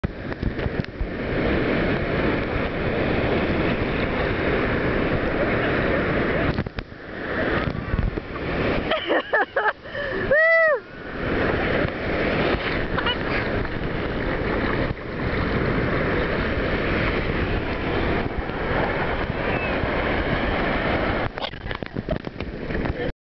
Ray's First Dip in the Ocean
My husband Ray's first venture into the ocean in the Gulf of Mexico at Isla Blanca Park on South Padre Island. He loved it! The surf is FUN! I also used my Dicapac waterproof camera cover.